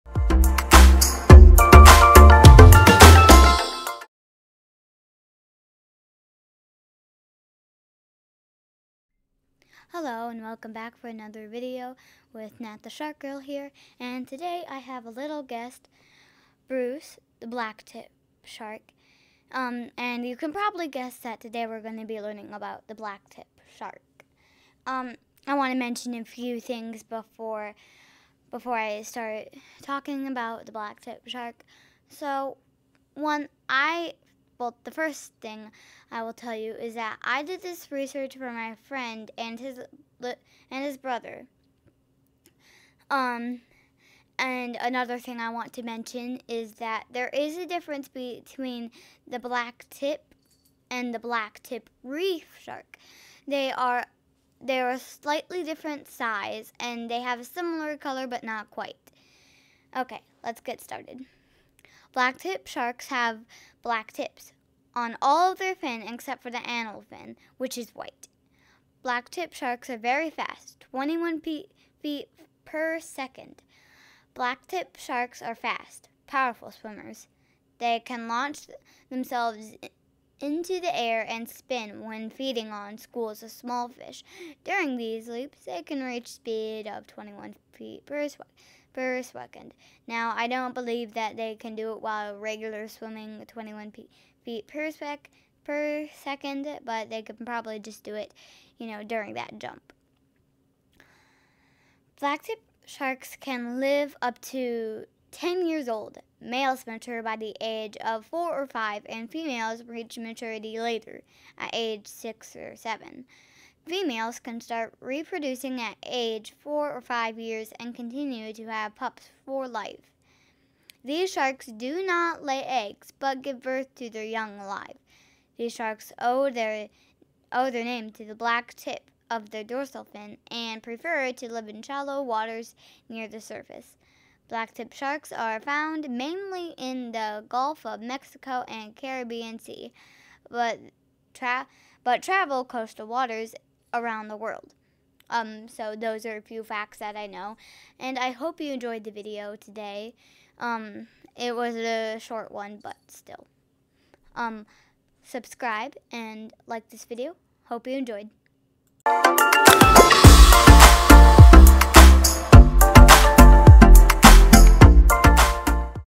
Natalie and Black Tip Sharks
My name is Natalie and today I am here to talk about Black Tip sharks - NOT to be confused with Black Tip Reef sharks!